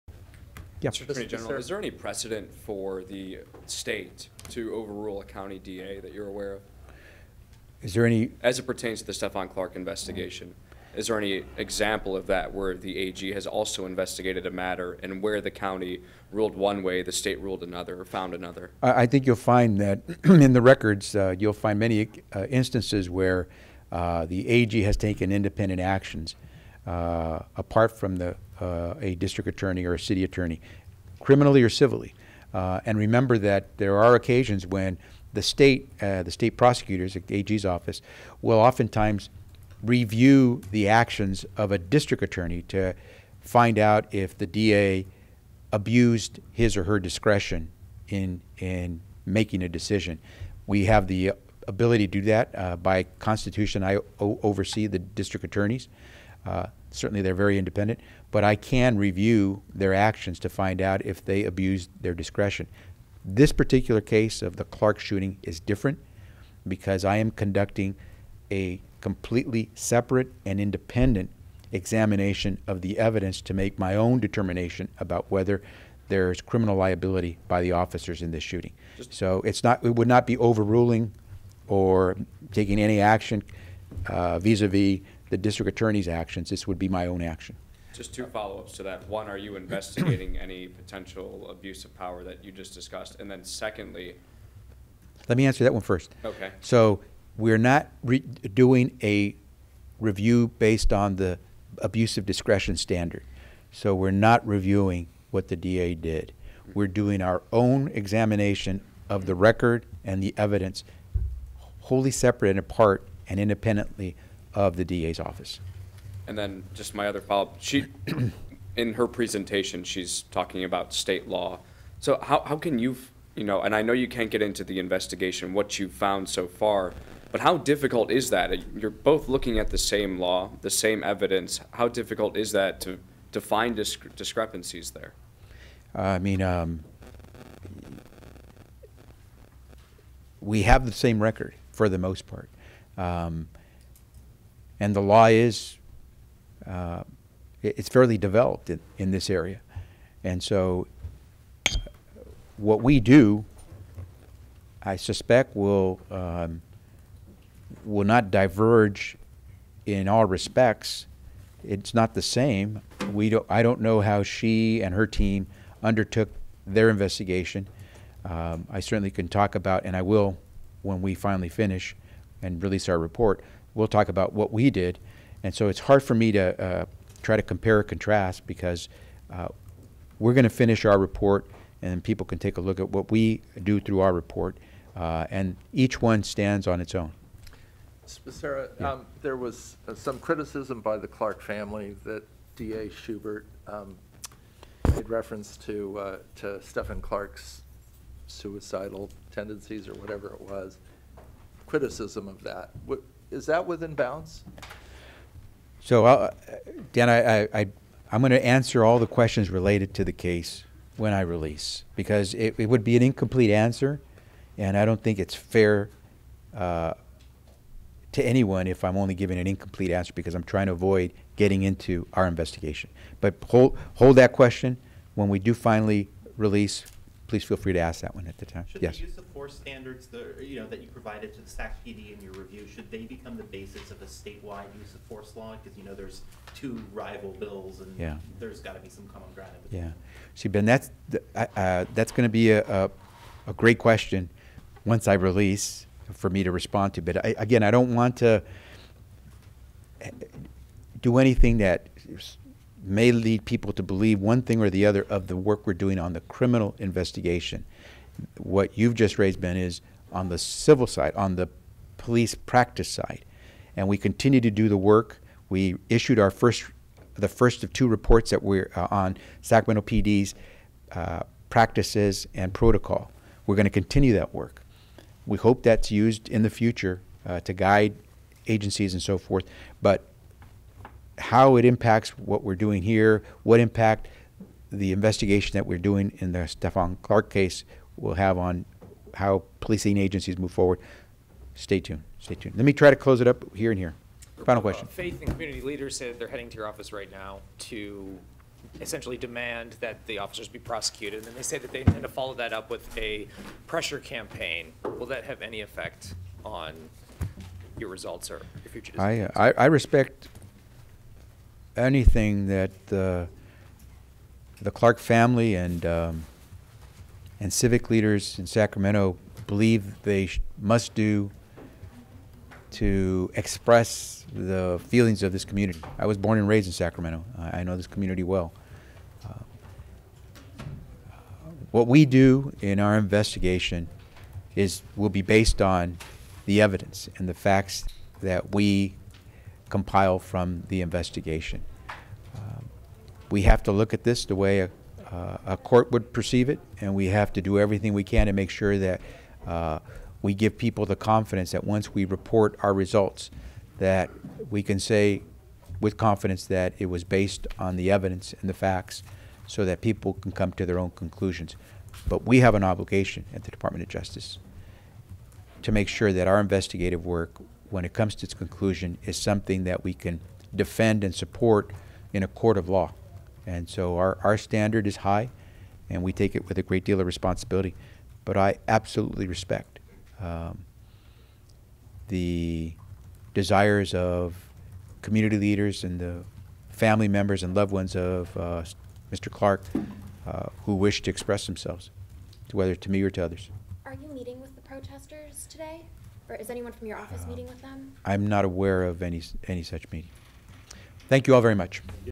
California AG Xavier Becerra responds to DA’s decision on Stephon Clark case | Extended Cut
California Attorney General Xavier Becerra spoke with reporters Monday morning about Sacramento District Attorney Anne Marie Schubert's decision to not file criminal charges against the police officers who shot and killed Stephon Clark.